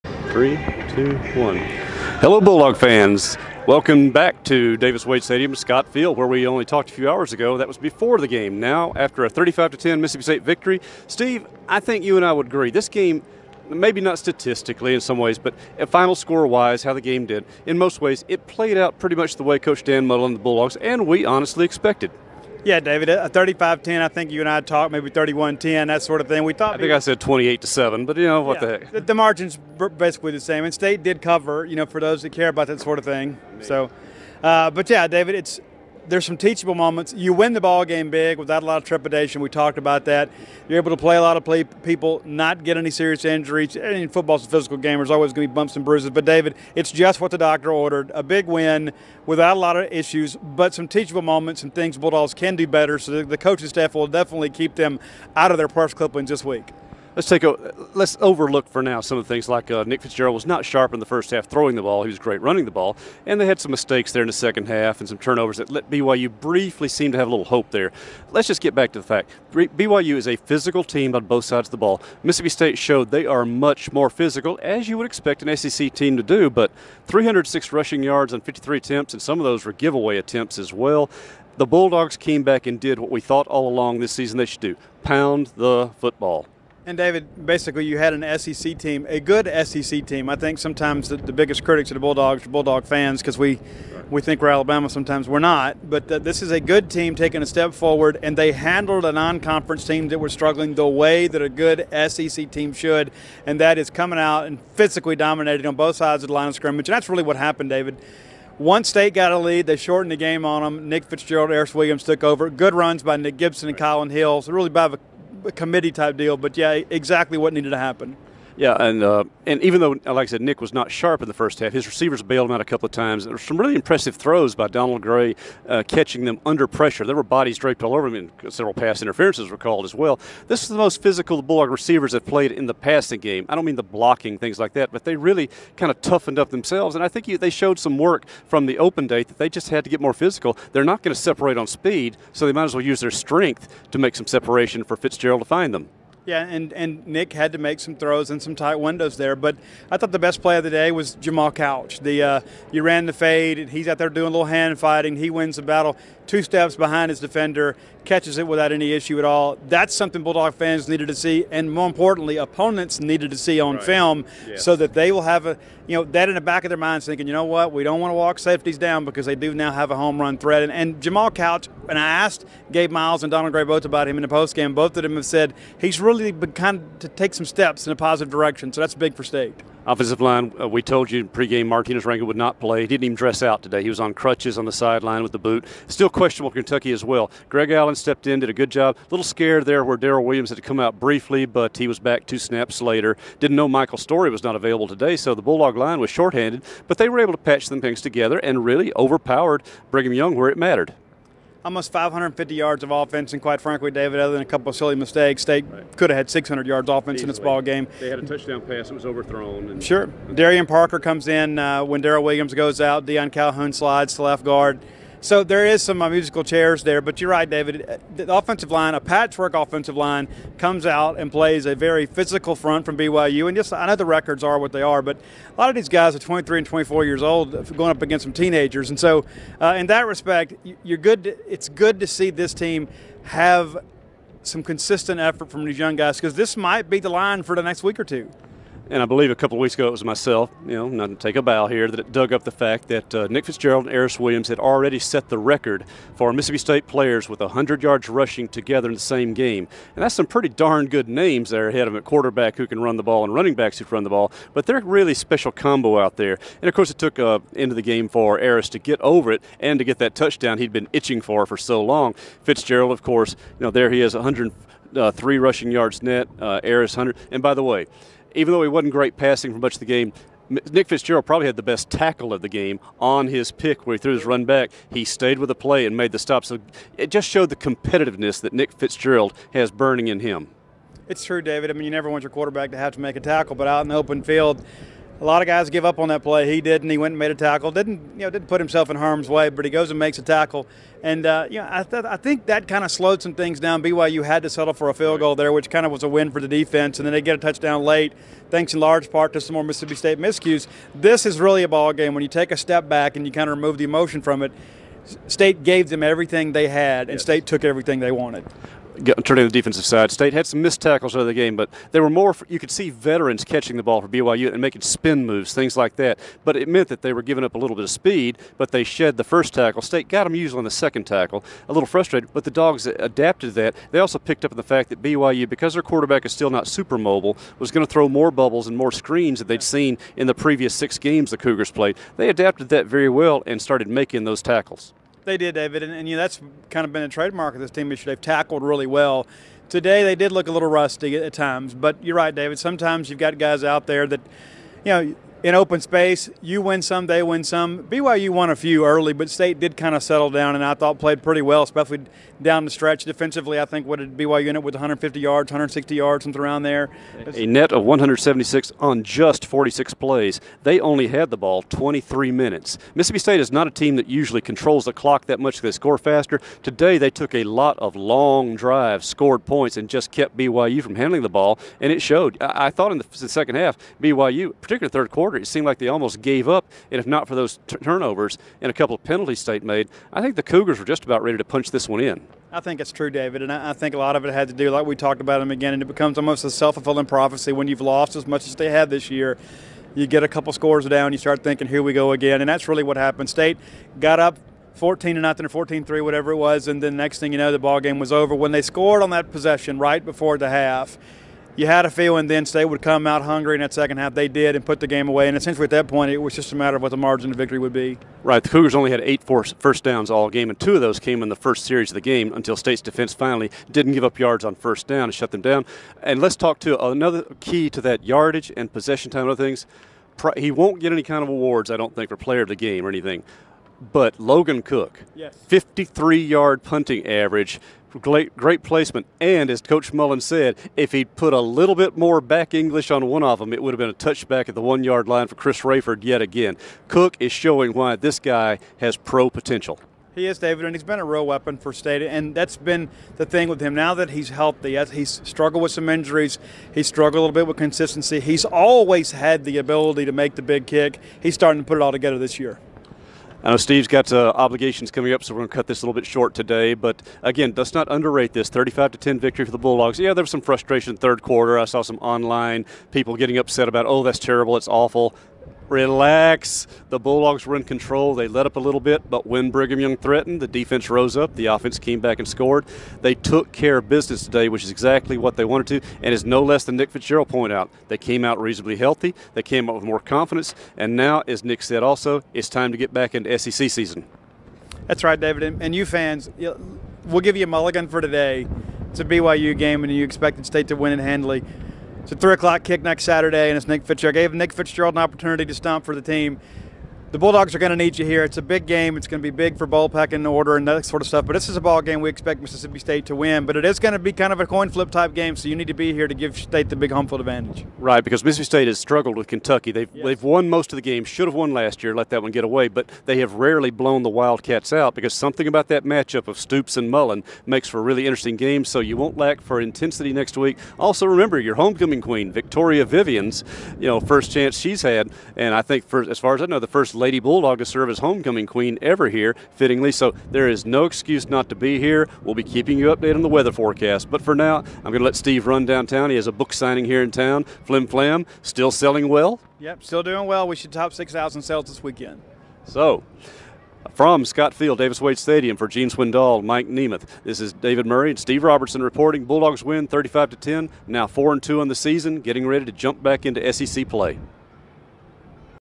BYU wrap up
David Murray and Steve Robertson recap Mississippi State's win over BYU